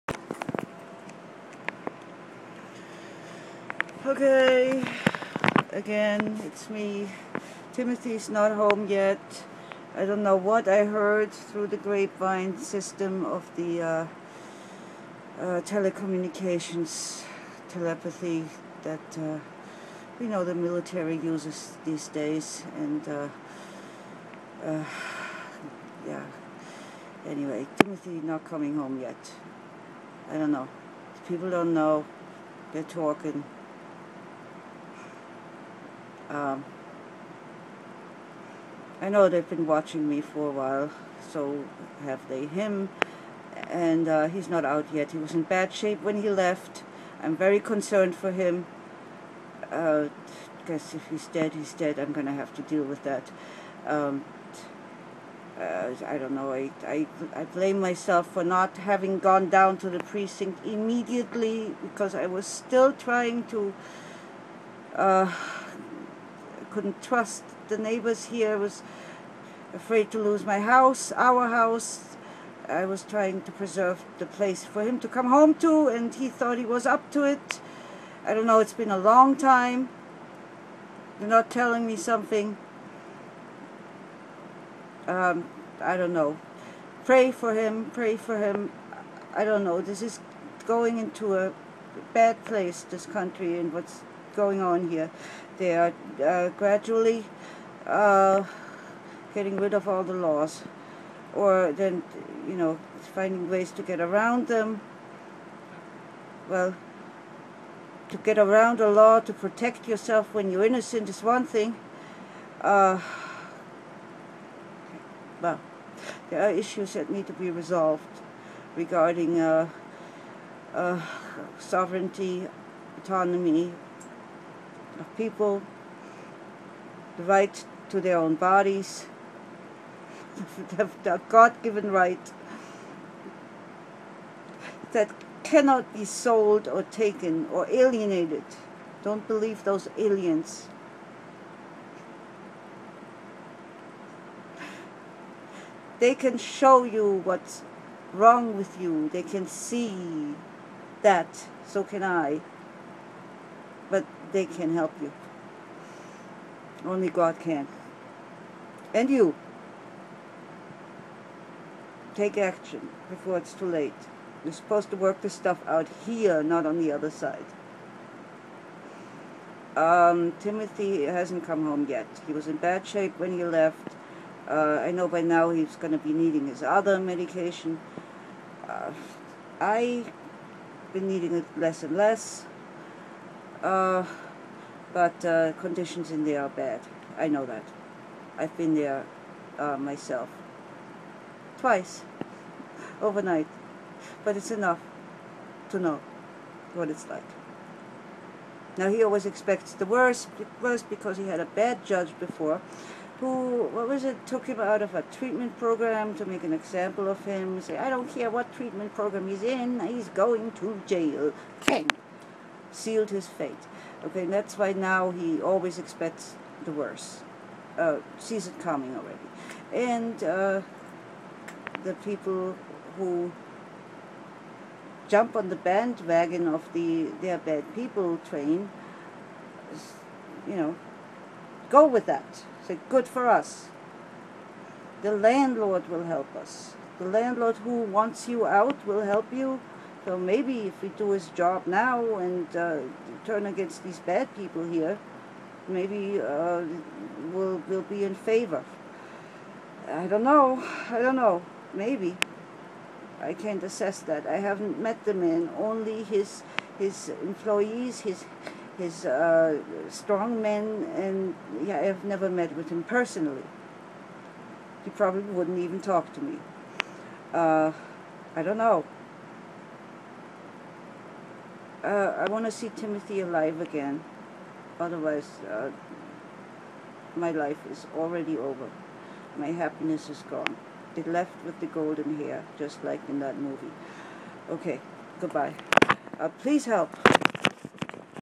Timothy still gone! Locked up on false charges!
With Timothy"s ex- friend"s help we checked on warrants in his name. There were  none. What I didn't  Know is that an "open complaint" is enough to arrest someone. Yerbal assault as we"ve been subjected to many times by the people who rented this rppm to us, a.k.a. harassment is not an arrestable offfense according to the arresting officer. There are some inconsistencies here. When he asked when the man last harassed me, the words failed me, too upset. Answer: Now, every time I attempt to talk to him about anythiing., every time he sees me. He has misrepresented things to us repeatedly, for once as to the Identity of the landlord, later admitted this. The notarized statement saus he has been threatened by the landlord with eviction unless he "get"s us people to leave." He tried " constructive eviction", that failed. Every time he fails he gets angrier at us, even though we advised him correctly to keep his lease, paid on time, were respectful & quiet with them.  They, however have not been with us for some time now. I have told him that I am willing to move just require time to do it. Paying him has not changed a thing, just left us less able to move. That was last month. He brought the construction guy. super, they all yelled at us tried to intimidate us into letting them "fix the room" & us oiut of it. Focus was on us having "nö privacy".  Isn't that what one pays rent for? The "alleged assault" was a light tap on her shoulderr to get her attention. He was in bad shape when he left, should have gone to hospital insteaf, netter than that place. In fact we just want to be left alone. If he dies from this who is going to bring him back? The legal system? I have to sneak in there like a thief to get my own food out of the fridge, Taking to peeing in bottles long time ago. easier. Feeling headpressure while the scalart waves are cooling me. Need to eat.